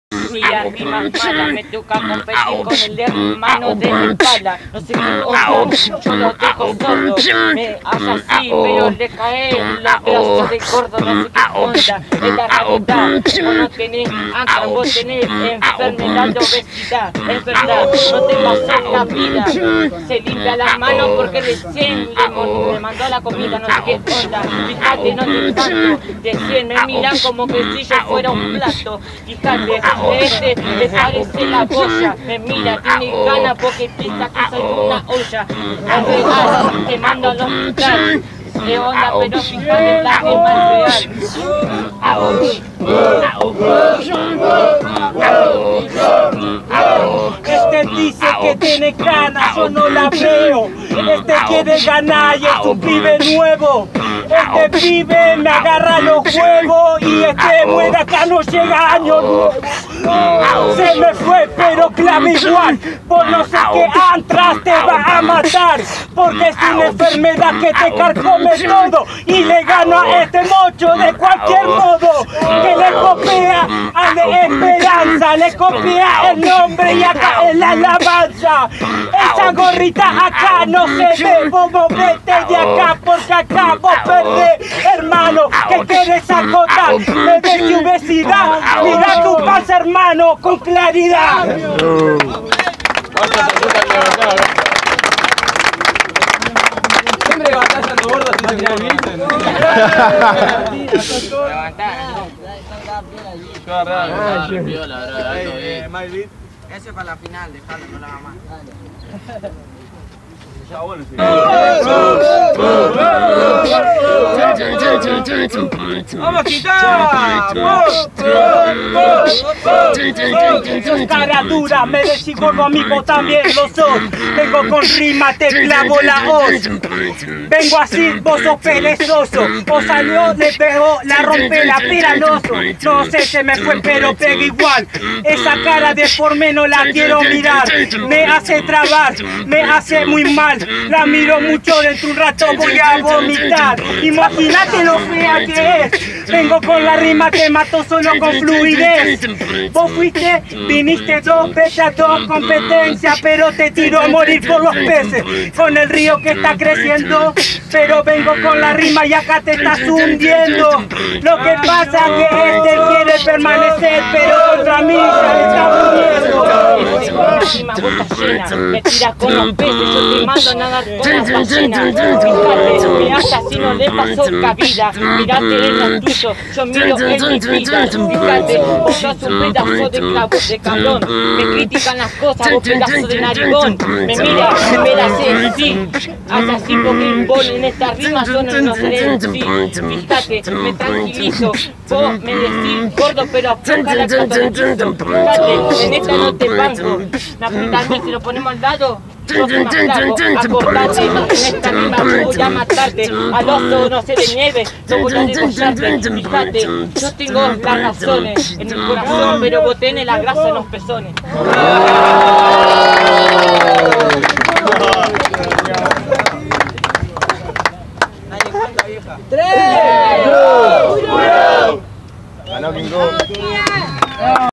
Prosser vs King Kong 8vo Rap Clique 2da Edicion
Bueno , Salio la Segunda edición de Rap Clique , se hace en la bajada del puente carretero (Santo Tomé)
Domingo 27 a las 17:30
1 vs 1 CON TEMÁTICA , Si hay replica es libre (SIN TEMÁTICA).
Compiten todos.
Jurados: Licho-Axel-Conec .